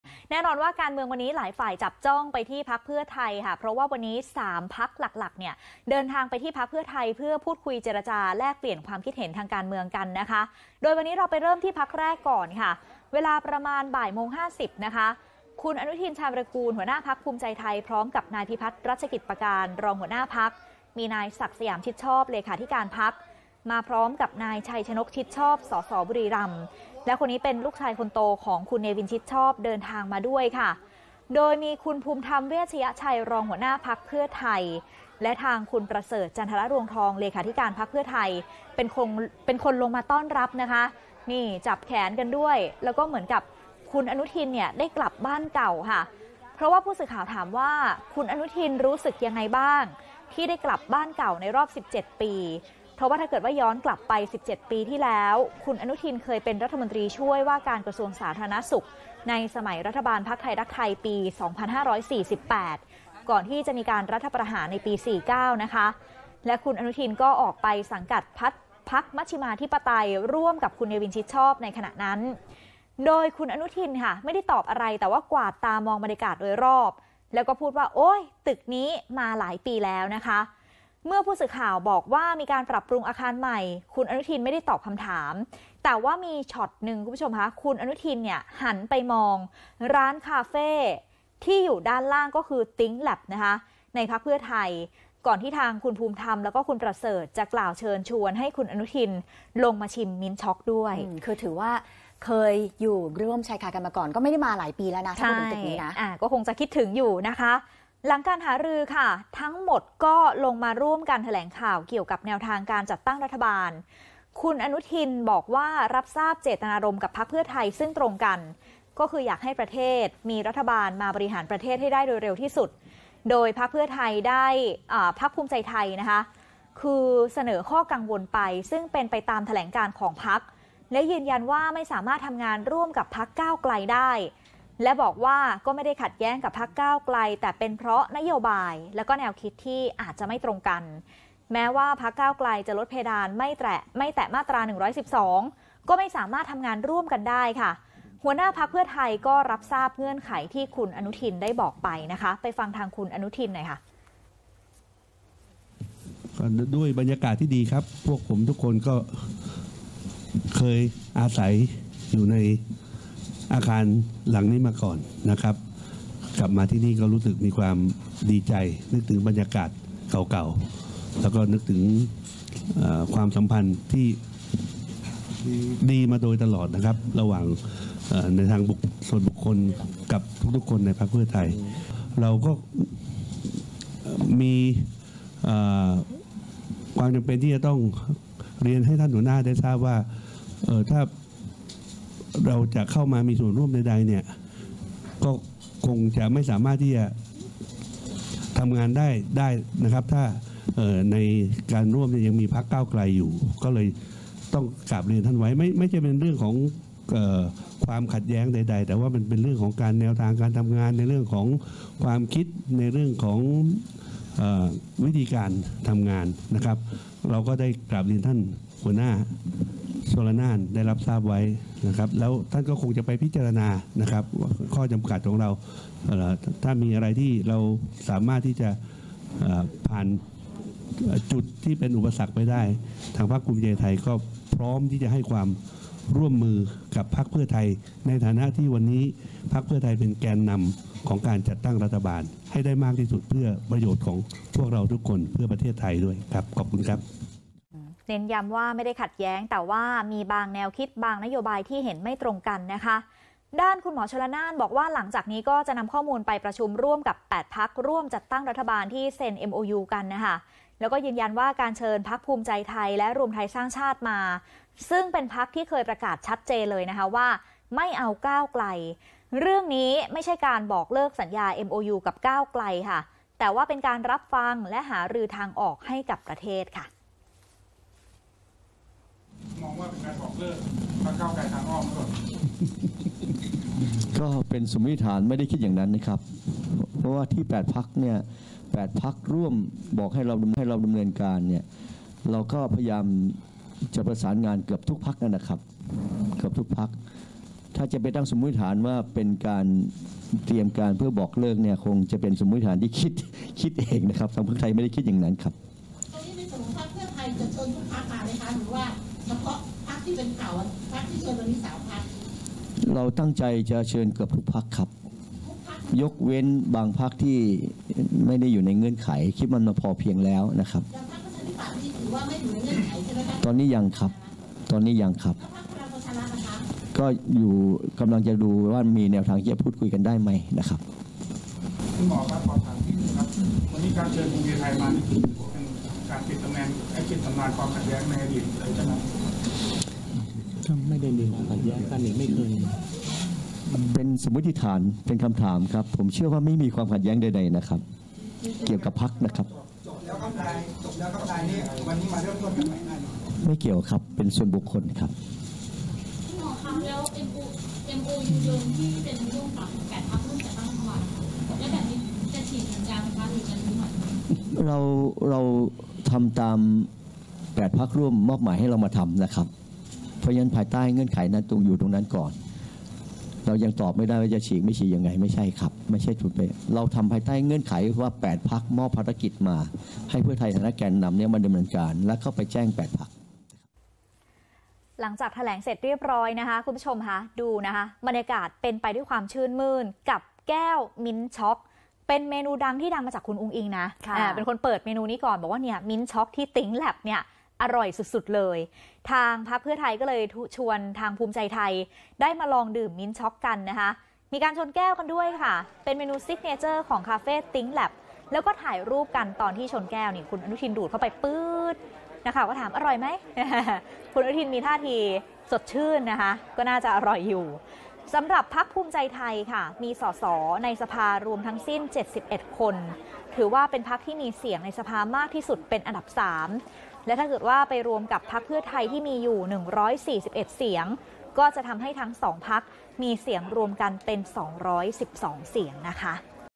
อนุทิน ชนแก้วมินต์ช็อก เพื่อไทย ชื่นมื่น หลังหารือประกาศจุดยืน ไม่เอาก้าวไกล
“อนุทิน” หัวหน้าพรรคภูมิใจไทย แถลงหารือร่วมกับ “นพ.ชลน่าน” หัวหน้าพรรคเพื่อไทย ถึงแนวทางจัดตั้งรัฐบาล ยืนยันภูมิใจไทยไม่สามารถทำงานกับก้าวไกลได้ เนื่องจากนโยบายและแนวคิดไม่ตรงกัน ก่อนกลับชนแก้วมินต์ช็อก เพื่อไทย ชื่นมื่น